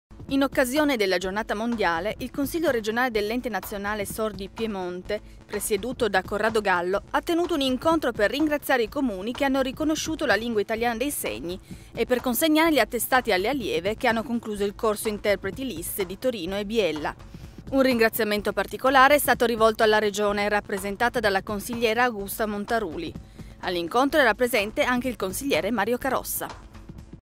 Giornata mondiale dei Sordi
In occasione della Giornata mondiale, il Consiglio regionale dell'Ente nazionale sordi Piemonte, presieduto da Corrado Gallo, ha tenuto un incontro per ringraziare i Comuni che hanno riconosciuto la Lingua italiana dei segni e per consegnare gli attestati alle allieve che hanno concluso il corso interpreti Lis di Torino e Biella. Un ringraziamento particolare è stato rivolto alla Regione, rappresentata dalla consigliera Augusta Montaruli. All'incontro era presente anche il consigliere Mario Carossa.